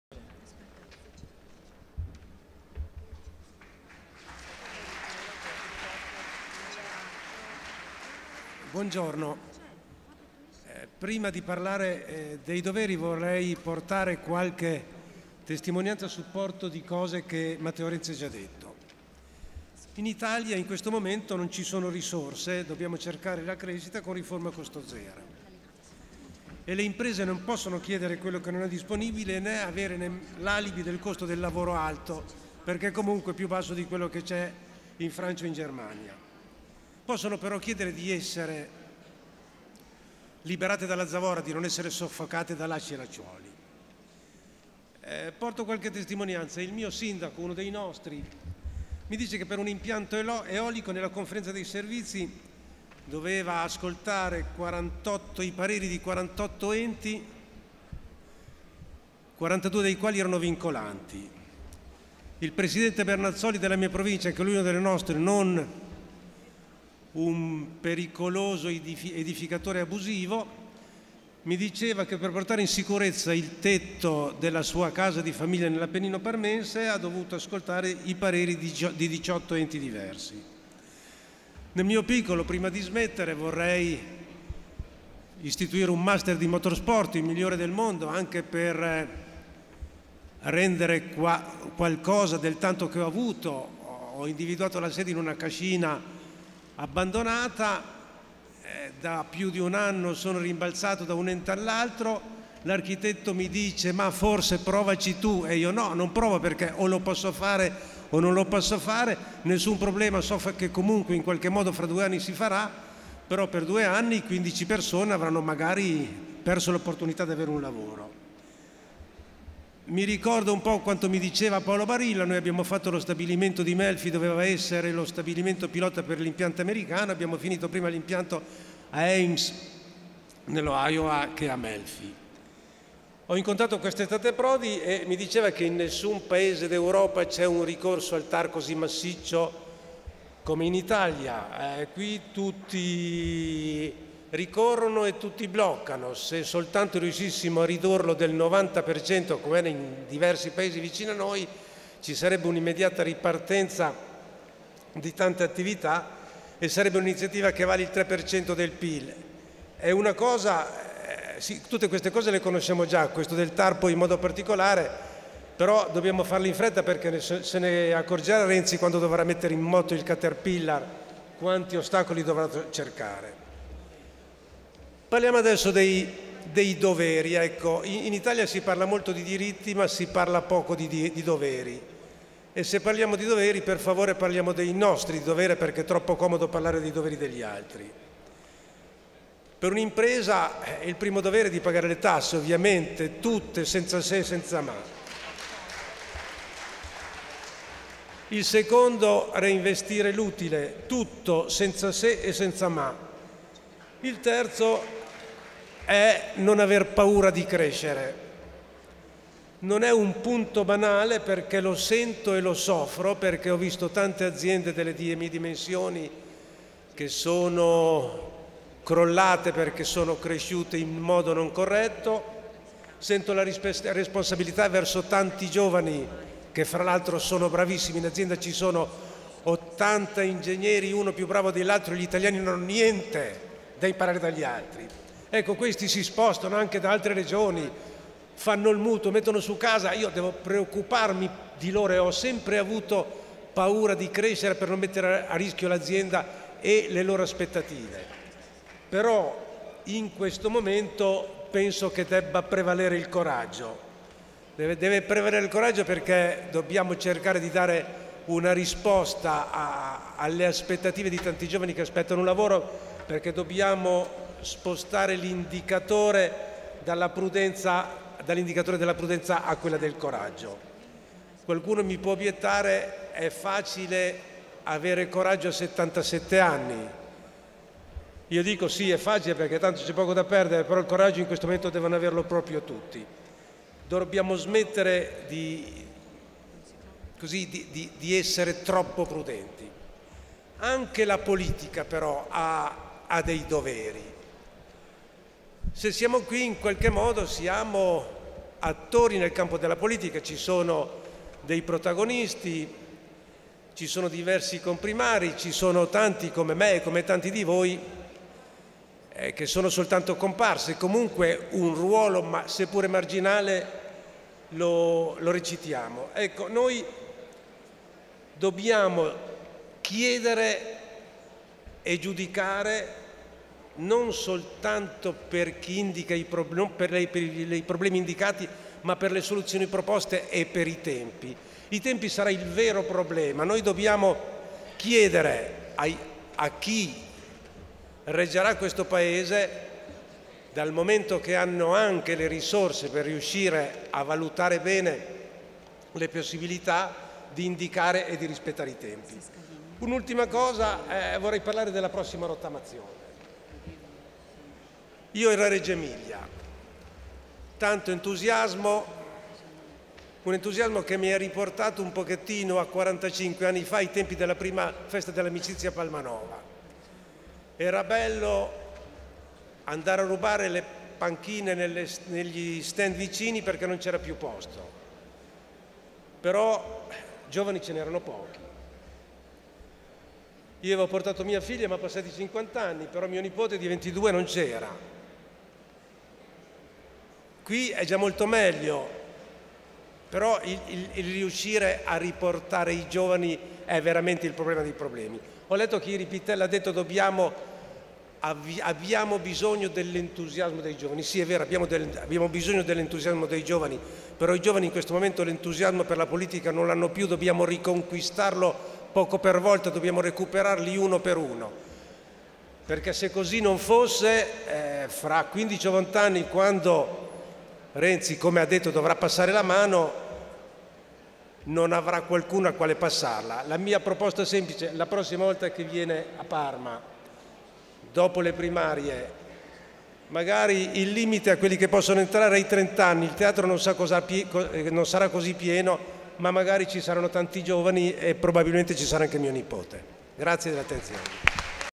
Gianpaolo Dallara a Leopolda13
L'intervento di Gianpaolo Dallara, imprenditore, a Leopolda13